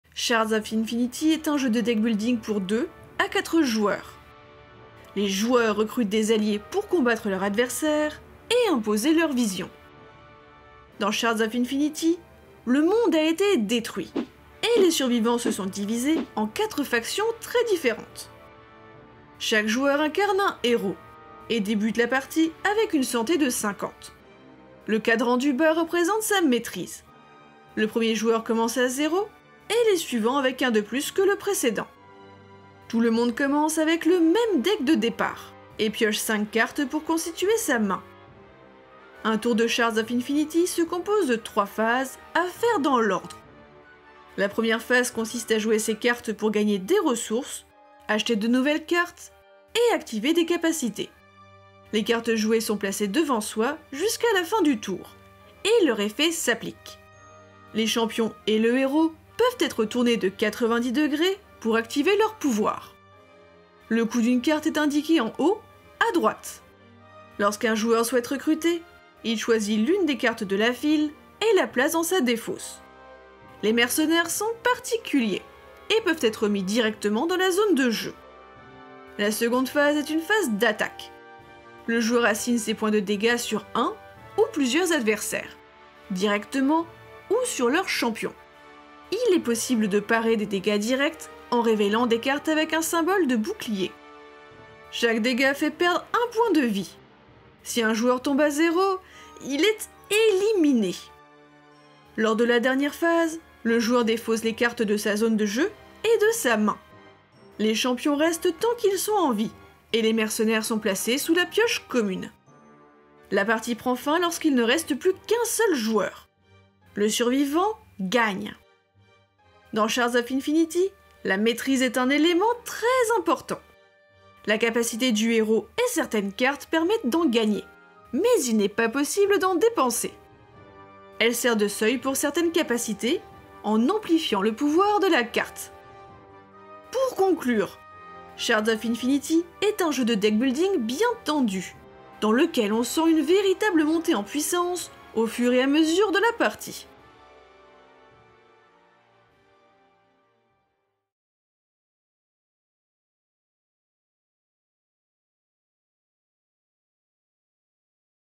Shards of Infinity - Présentation du jeu
Shards of Infinity est un jeu de deckbuilding pour 2 à 4 joueurs. Les joueurs recrutent des alliés pour combattre leurs adversaires et imposer leur vision.

Informations sur le jeu :
●  30 minutes
● Auteurs : Gary Arant, Justin Gary
● Illustrateur : Aaron Nakahara
● Editeur : IELLO